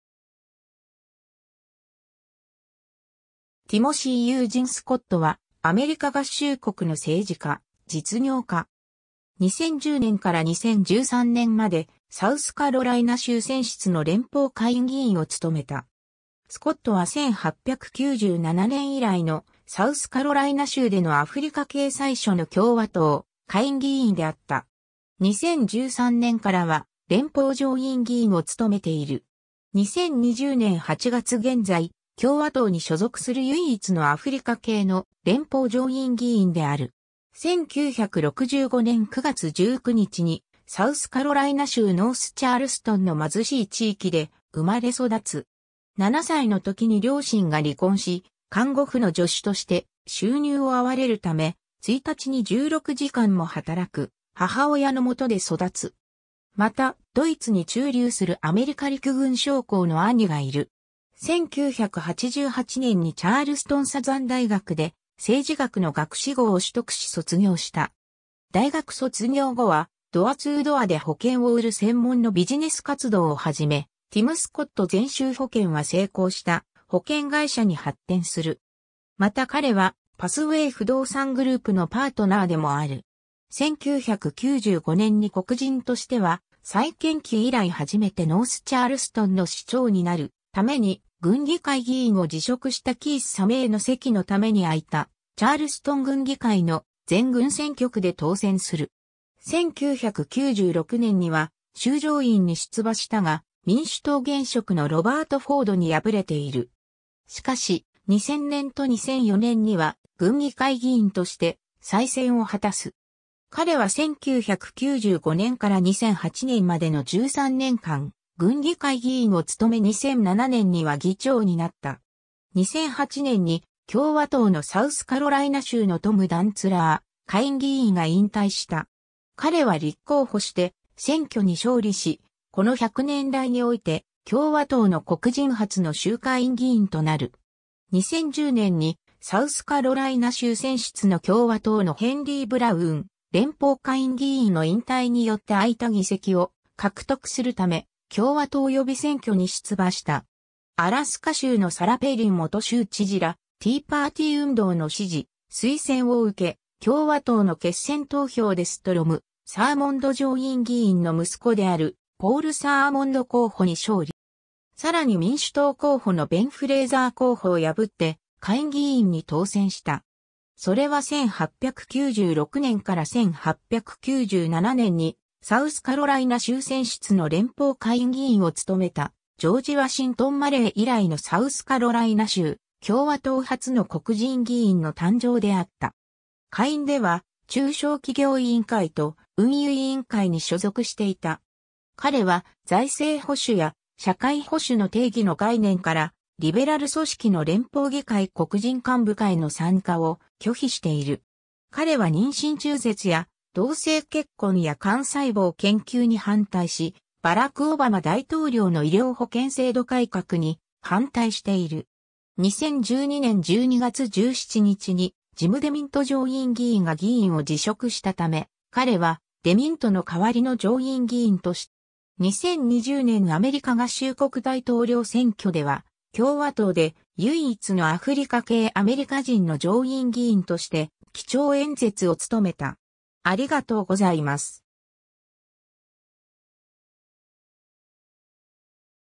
ティム・スコット
ティモシー（ティム）・ユージン・スコット（Timothy Eugene"Tim" Scott、1965年9月19日 -） は、アメリカ合衆国の政治家・実業家。
2010年から2013年まで、サウスカロライナ州選出の連邦下院議員を務めた。
スコットは1897年以来のサウスカロライナ州でのアフリカ系最初の共和党下院議員であった。
2013年からは連邦上院議員を務めている。
2020年8月現在、共和党に所属する唯一のアフリカ系の連邦上院議員である。
 1965年9月19日にサウスカロライナ州ノースチャールストンの貧しい地域で生まれ育つ。
7歳の時に両親が離婚し、看護婦の助手として収入を合われる為、1日に16時間も働く母親の元で育つ。
またドイツに駐留するアメリカ陸軍将校の兄がいる。
 1988年にチャールストン・サザン大学で政治学の学士号を取得し卒業した。
大学卒業後はドアツードアで保険を売る専門のビジネス活動を始め、ティム・スコット全州保険（Tim Scott Allstate Insurance）は成功した保険会社に発展する。
また彼はパスウェイ不動産グループのパートナーでもある。
 1995年に黒人としては再建期以来初めてノースチャールストンの市長になる為に郡議会議員を辞職したキース・サメイの席の為に空いたチャールストン郡議会の全郡選挙区で当選する。
 1996年には州上院に出馬したが、民主党現職のロバート・フォードに敗れている。
しかし、2000年と2004年には郡議会議員として再選を果たす。
、彼は1995年から2008年までの13年間、郡議会議員を務め2007年には議長になった。
2008年に共和党のサウスカロライナ州のトム・ダンツラー下院議員が引退した。
彼は立候補して選挙に勝利し、この100年来において共和党の黒人初の州下院議員となる。
 2010年にサウスカロライナ州選出の共和党のヘンリー・ブラウン連邦下院議員の引退によって空いた議席を獲得する為、共和党予備選挙に出馬した。
アラスカ州のサラ・ペイリン元州知事らティーパーティー運動の支持・推薦を受け、共和党の決選投票でストロム・サーモンド上院議員の息子であるポール・サーモンド候補に勝利。
さらに民主党候補のベン・フレイザー候補を破って下院議員に当選した。
それは1896年から1897年にサウスカロライナ州選出の連邦下院議員を務めたジョージ・ワシントン・マレー以来のサウスカロライナ州共和党初の黒人議員の誕生であった。
下院では、中小企業委員会と運輸委員会に所属していた。
彼は財政保守や社会保守の定義の概念から、リベラル組織の連邦議会黒人幹部会の参加を拒否している。
彼は妊娠中絶や同性結婚や幹細胞研究に反対し、バラク・オバマ大統領の医療保険制度改革に反対している。
 2012年12月17日にジム・デミント上院議員が議員を辞職した為、彼はデミントの代わりの上院議員として当時のサウスカロライナ州知事であったニッキー・ヘイリーによって任命された。
 2020年アメリカ合衆国大統領選挙では、共和党で唯一のアフリカ系アメリカ人の上院議員として基調演説を務めた。